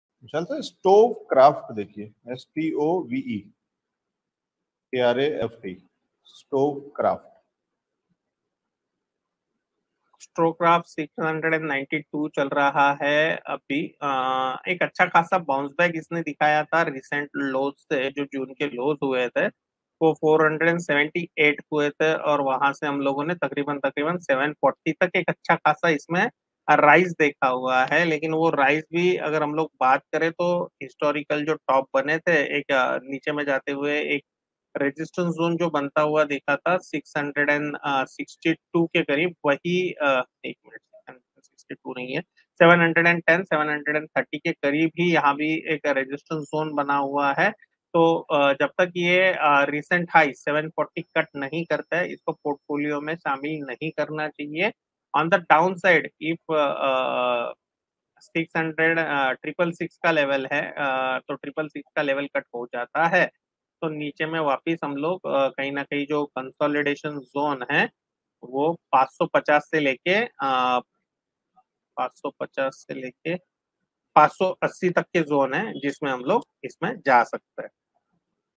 Stove Kraft Ltd. के शेयर में क्या करें? Expert Opinion by Vishal Wagh
जानिए हमारे Stock Market Expert Vishal Wagh की राय |

Analysis of: Stove Kraft Ltd.

Shows on StockMarket equity trading investment commodity personalfinance tax cryptocurrency blockchain reviews stovekraft marketanalysis startups policyreviews nifty sensex and much more.